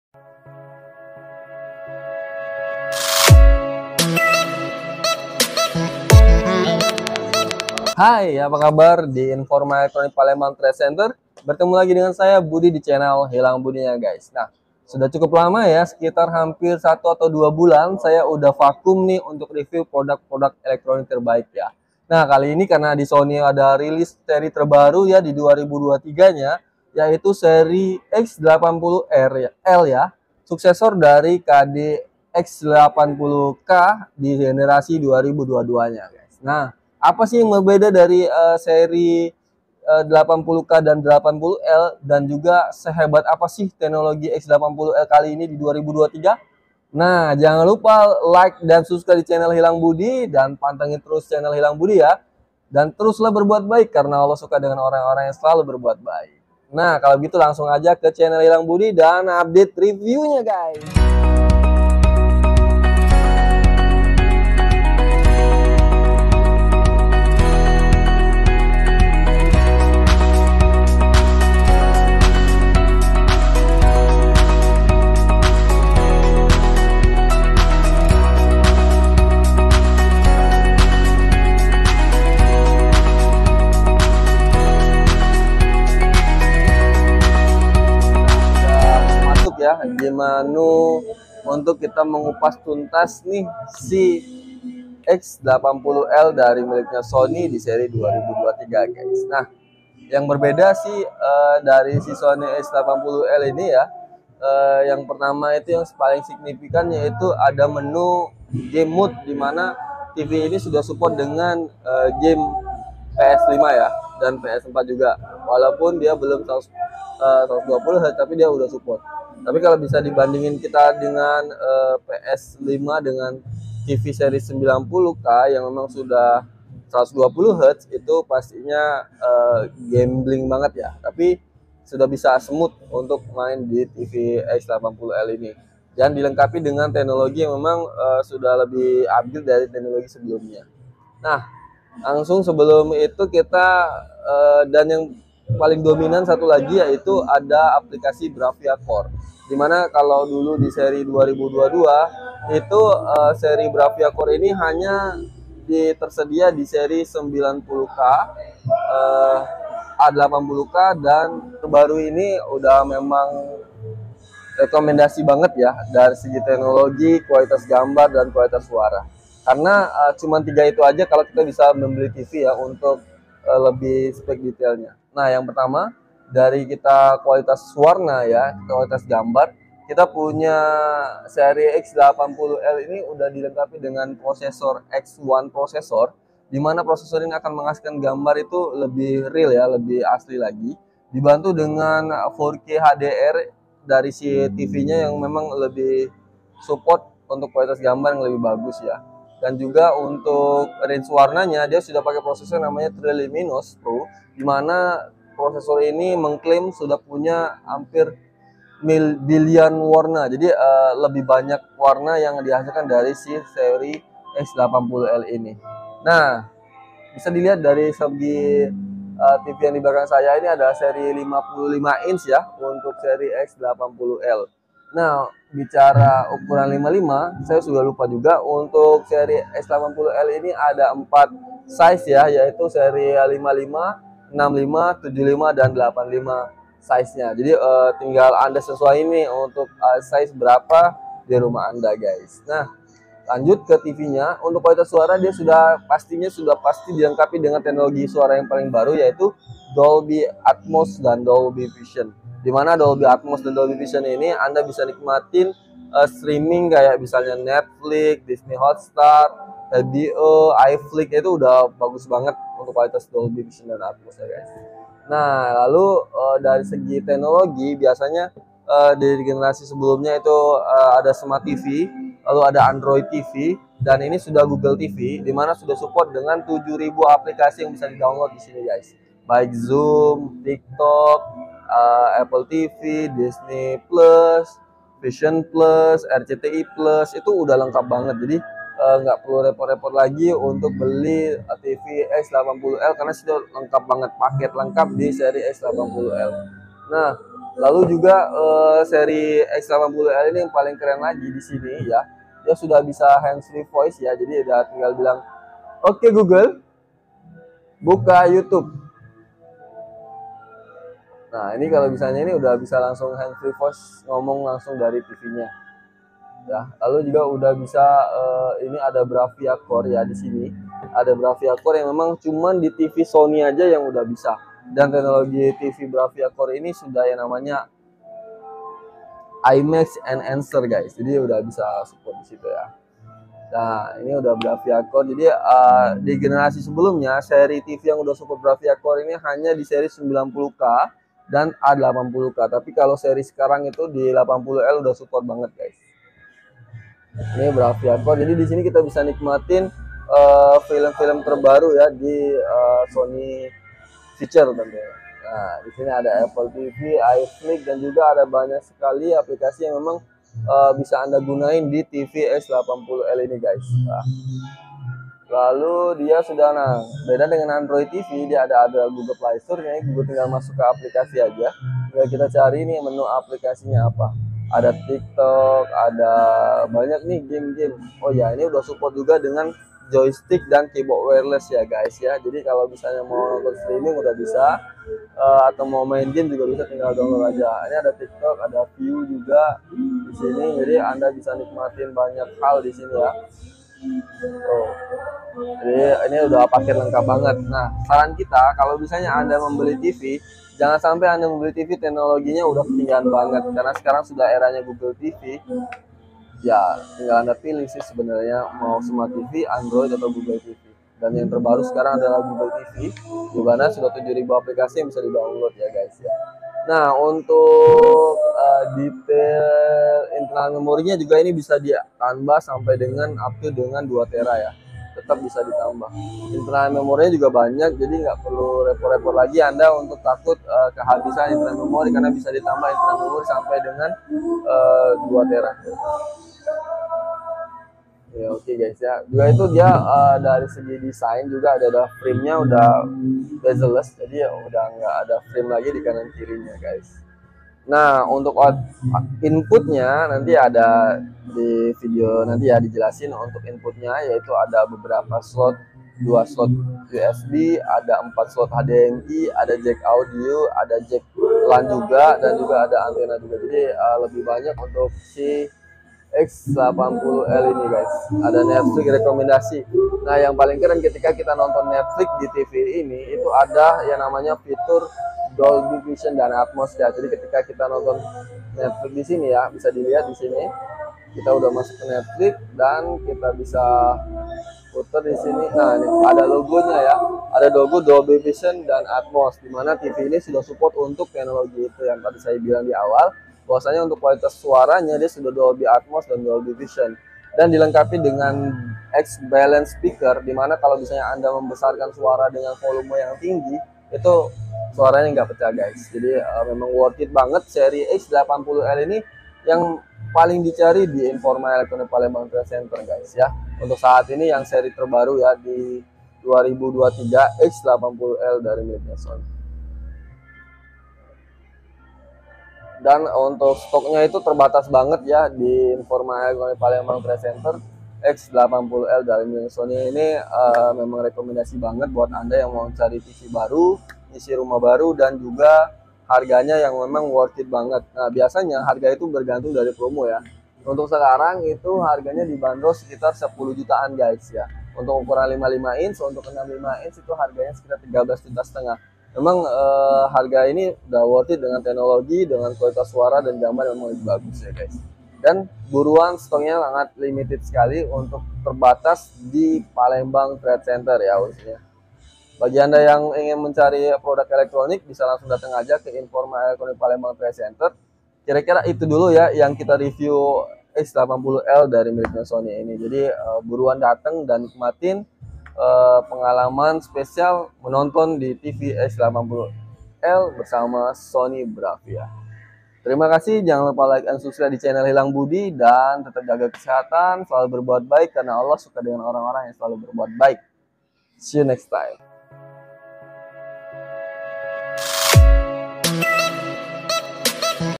review terbaru yang terbaik X80L 2023
seri terbaru 2023
X80L
Ya seri di seri 2023 dari miliknya Sony ini dilengkapi dengan teknologi yang memang sangat signifikan berbeda banget misteri 2022 nah mau tahu nih Seperti apa teknologi yang terbaru di Sony 2023 yuk langsung aja ke video berikut ini